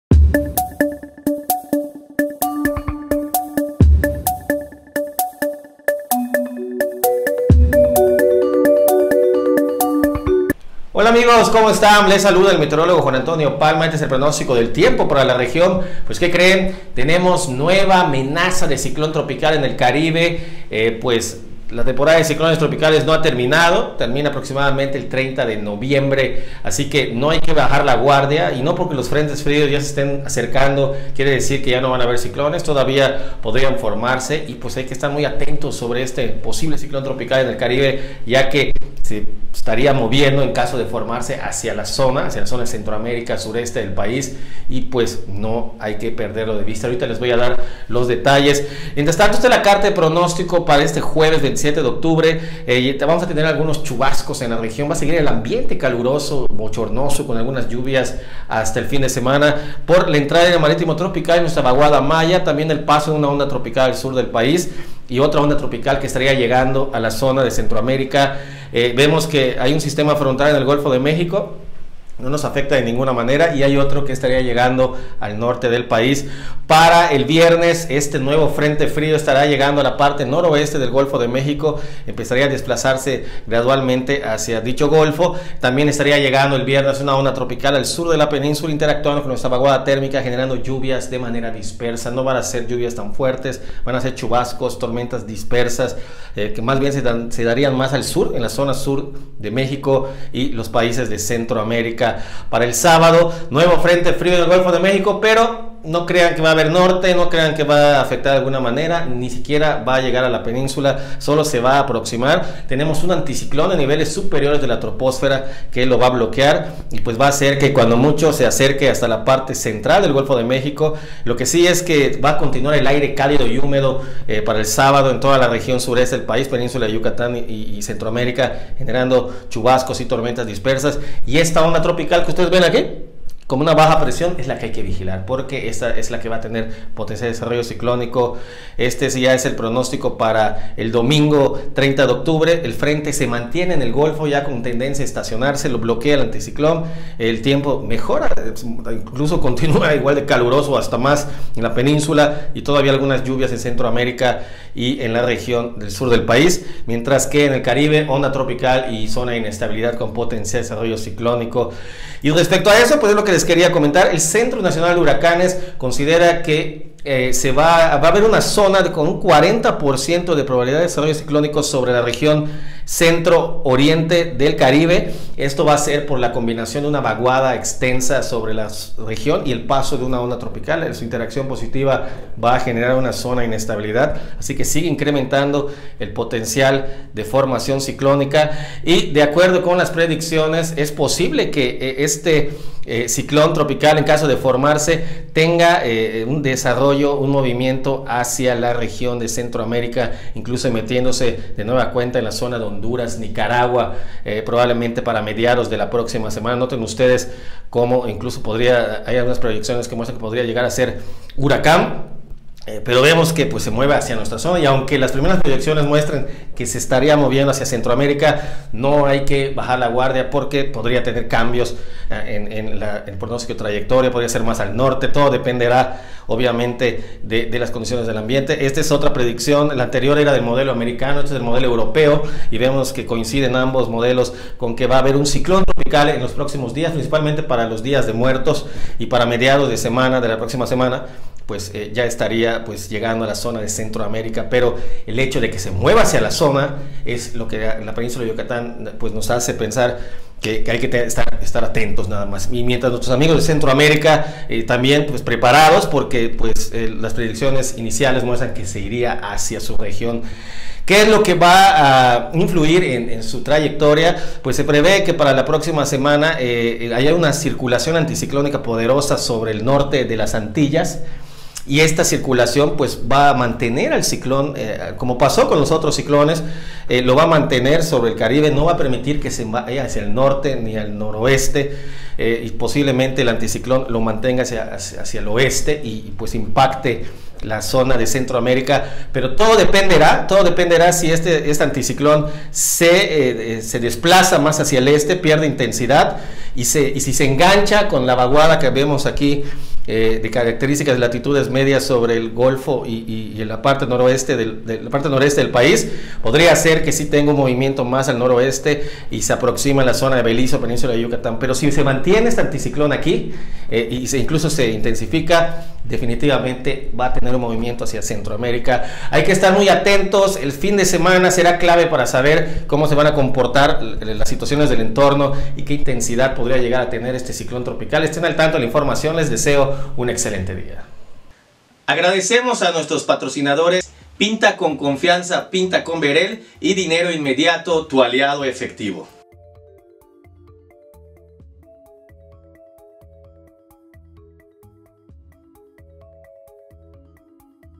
Amenaza de formación ciclónica en el mar Caribe
Estén muy atentos en Centroamérica y en la Península de Yucatán no bajen la guardia.

Pronóstico para hoy 27 de octubre 2022